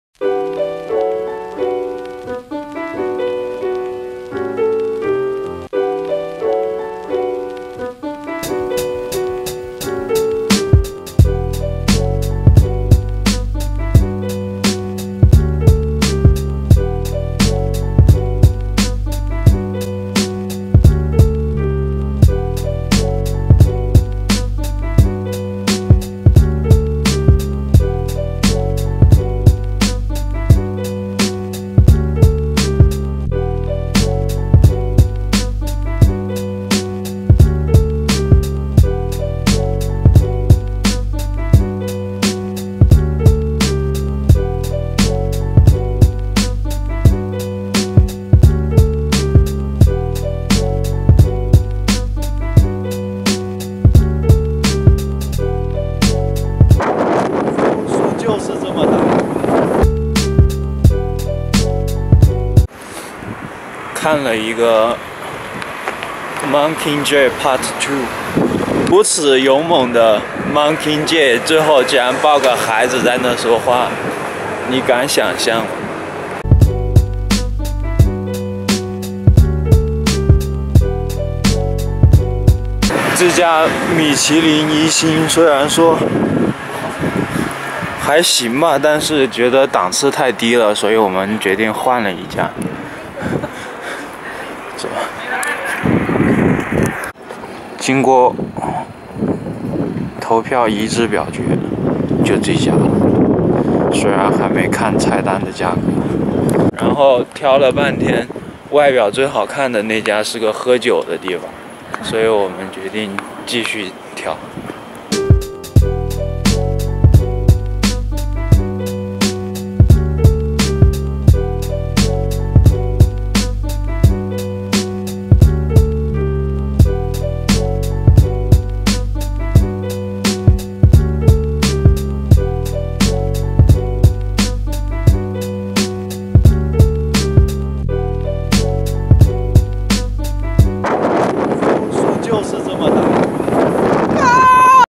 One day chillout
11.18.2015 after saw the premiere of final part of Hunger Game, got one of the best dinner in Netherland!!